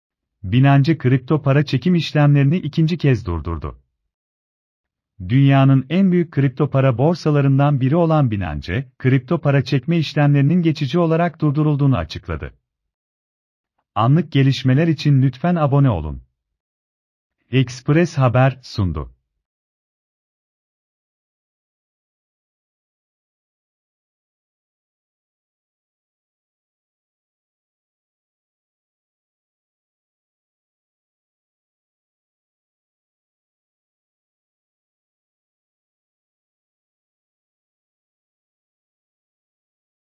Son dakika... Binance kripto para çekim işlemlerini ikinci kez durdurdu
Dünyanın en büyük kripto para borsalarından biri olan Binance, kripto para çekme işlemlerinin geçici olarak durdurulduğunu açıkladı.